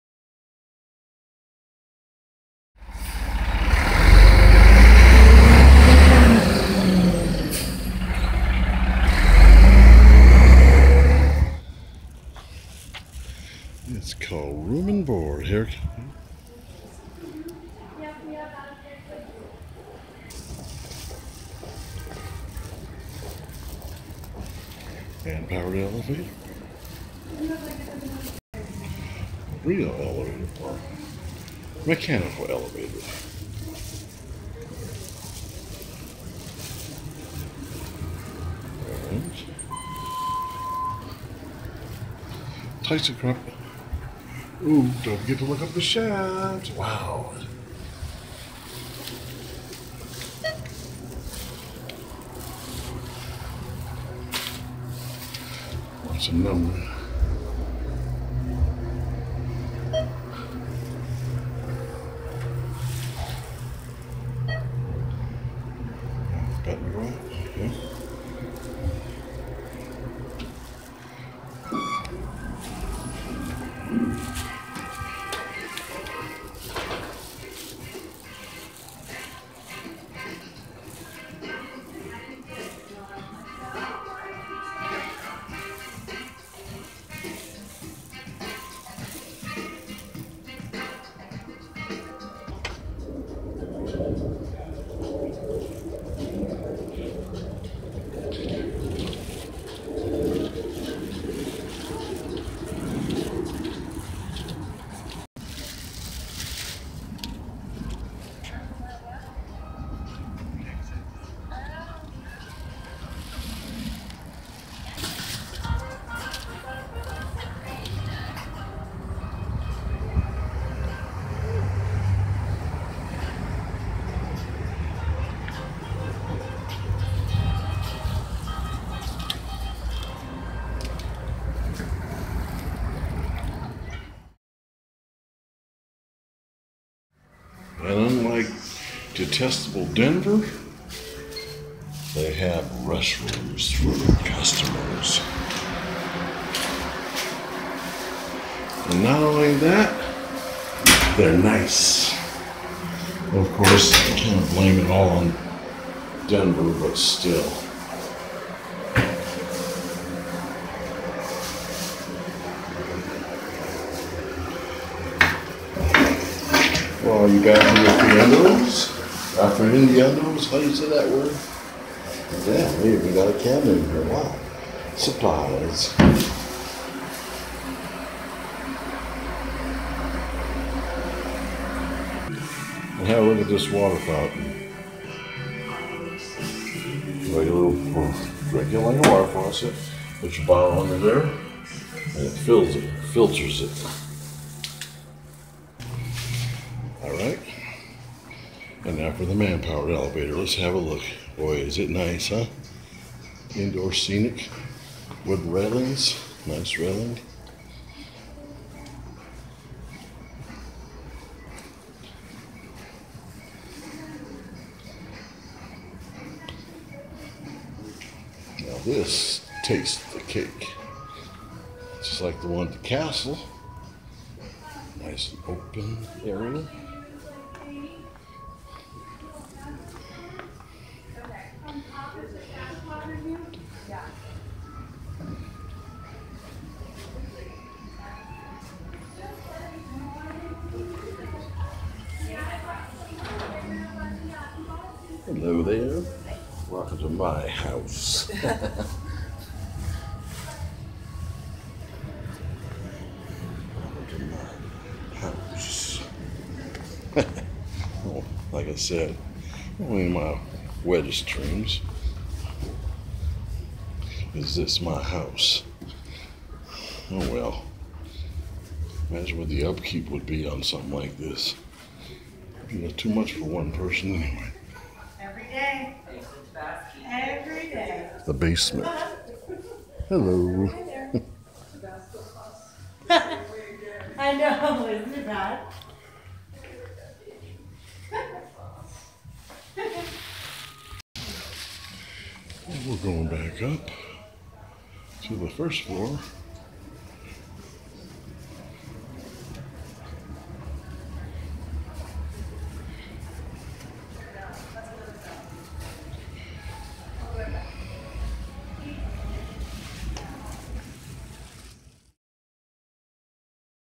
Mini Tour of Room & Board With Elevators & Bathroom from ‎October ‎27, ‎2017
I originally recorded this home furnishing and decoration store back in 2010 or so but back then it was a much smaller store with only a lower level.  That video has long since been lost.  Here is the store in 2017 with it's expanded update with three gorgeous levels with an elevator, a glorious "man-powered" elevator  and even something for the bathroom aficionados.